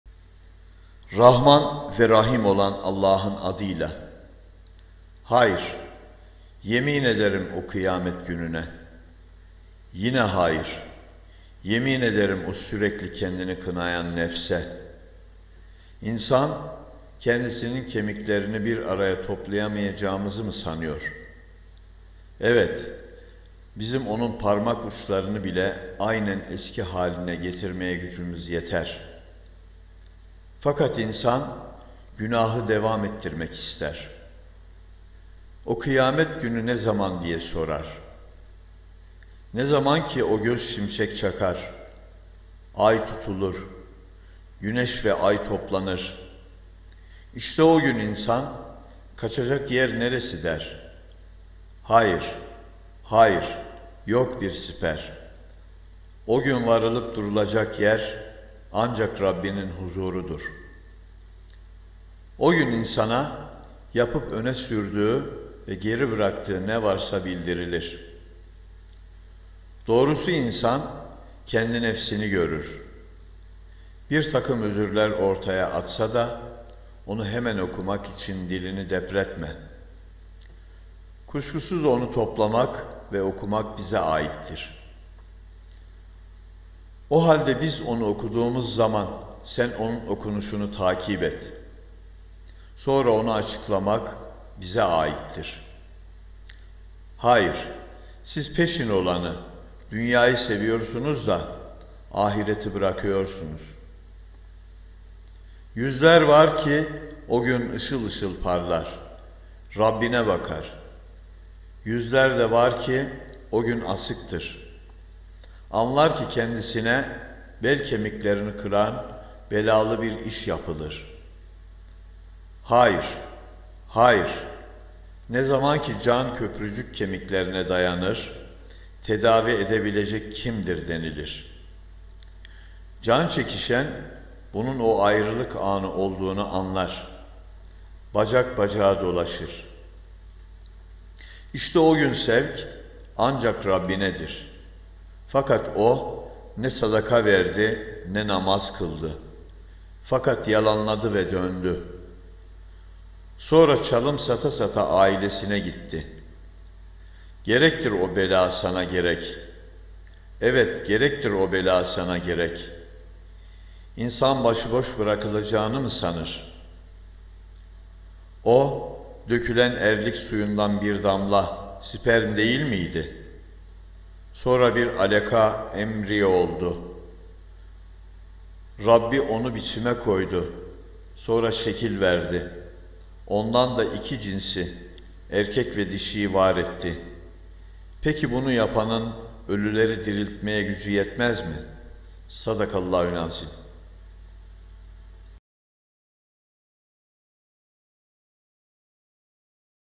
KIYAMET SURESİ MEALİ DİNLE KUR ANİ KERİM MEALİ
KUR AN MEALİ  KIYAMET SUREİ ŞERİFİ TÜRKÇE AÇIKLAMASI SESLİ  NÜZUL SIRASINA GÖRE TAMAMI TEK PARÇA HALİNDE SESLENDİRME  LİSTEN TO DESCRIPTION OF THE SURAH KIYAMET
OKUYAN SESLENDİREN
YILDIRIM ŞİMŞEK